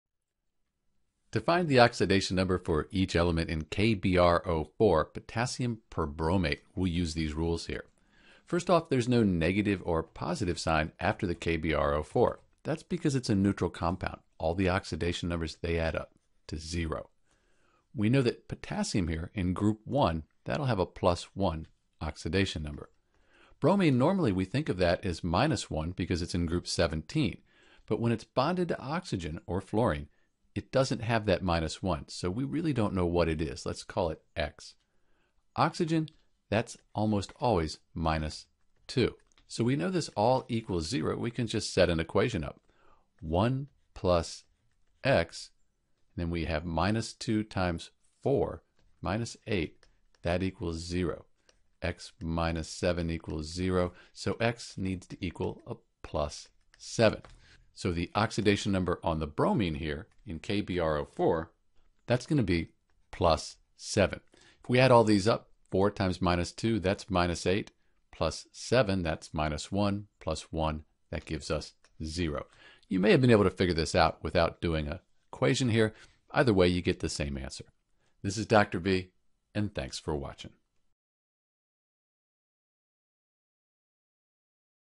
How to find the Oxidation Number for Br in KBrO4 (Potassium Perbromate)
To find the correct oxidation state of Br in KBrO4 (Potassium Perbromate), and each element in the compound, we use a few rules and some simple math.

First, since the KBrO4 doesn’t have an overall charge (like NO3- or H3O+) we could say that the total of the oxidation numbers for KBrO4 will be zero since it is a neutral compound.

We write the oxidation number (O.N.) for elements that we know and use these to figure out oxidation number for Br.

Here it is bonded to element symbol so the oxidation number on Oxygen is -2. Using this information we can figure out the oxidation number for the element Br in KBrO4.